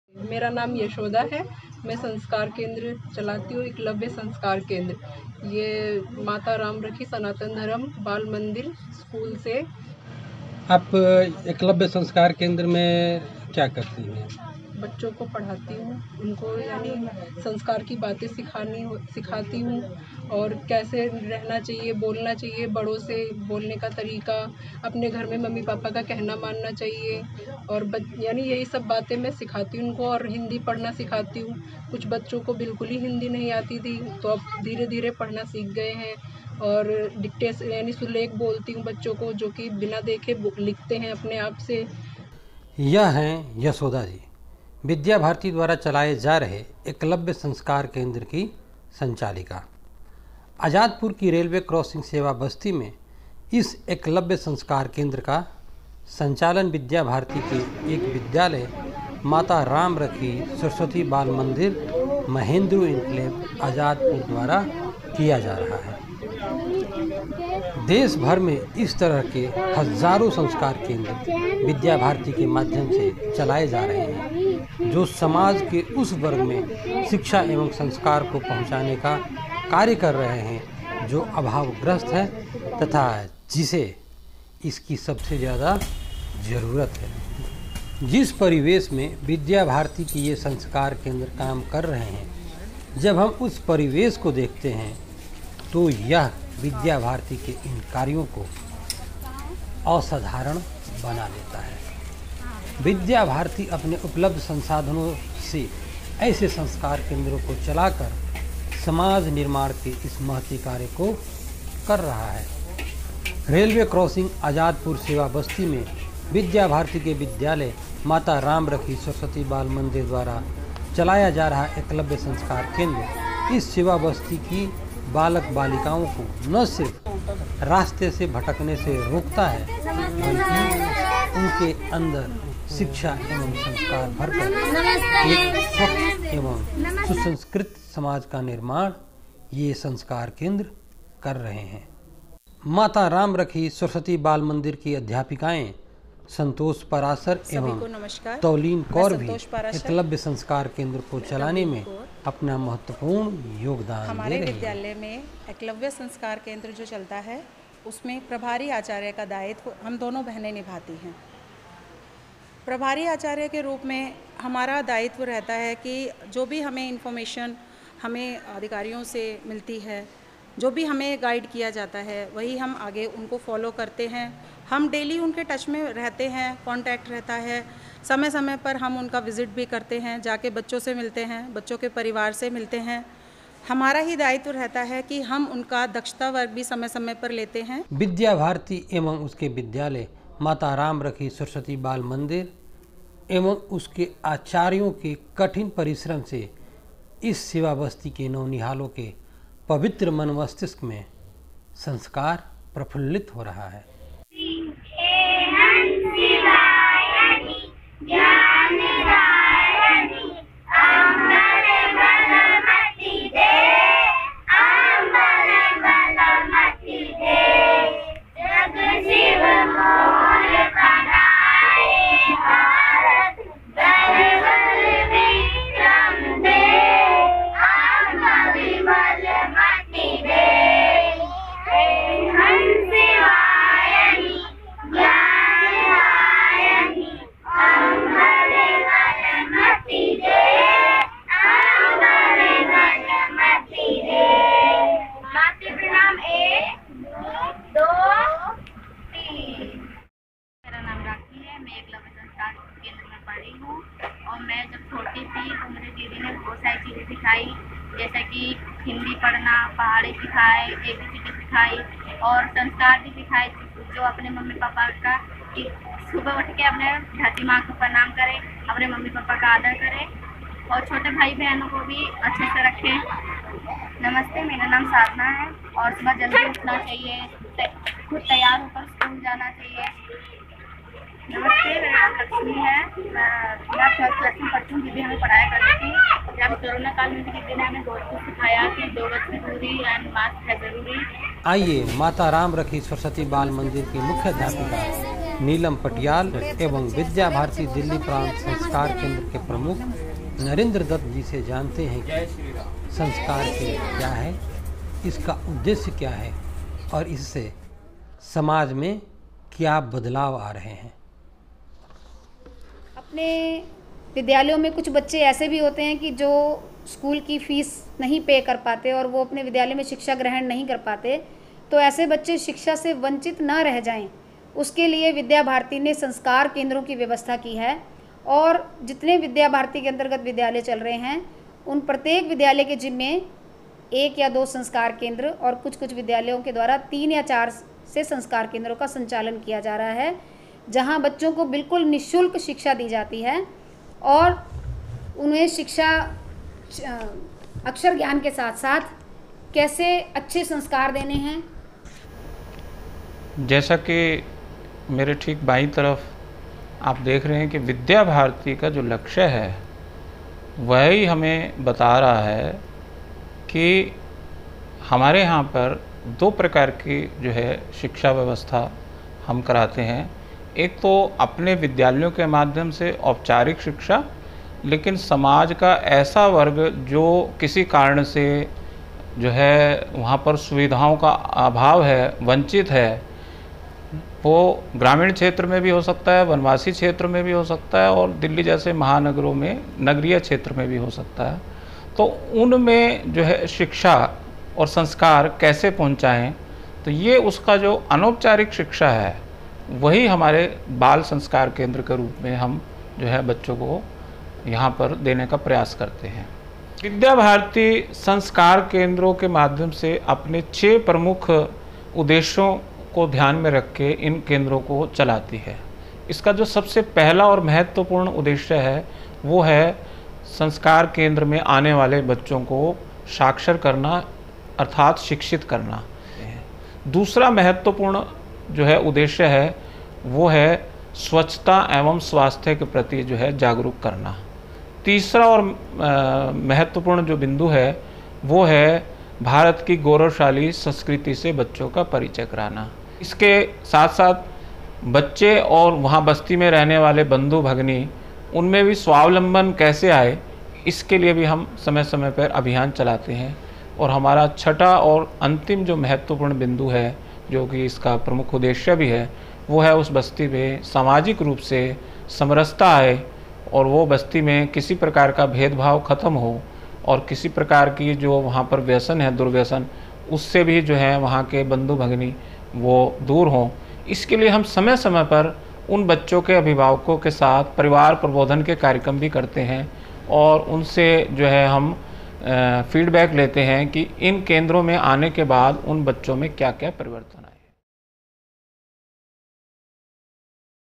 बच्चों को शिक्षित एवं सुसंस्कृत बनाते Vidya Bharati के Sanskar Kendra
दिल्ली के आजादपुर के रेलवे कॉलोनी सेवा बस्ती में विद्या भारती के विद्यालय माता रामरखी सरस्वती बाल मन्दिर, महेंद्रू एन्क्लेव द्वारा चलाया जा रहा एकलव्य संस्कार केंद्र सेवा बस्ती के बालक बालिकाओं के पवित्र मन मस्तिष्क में शिक्षा एवं संस्कार भरने का उत्तम कार्य कर रहे हैं।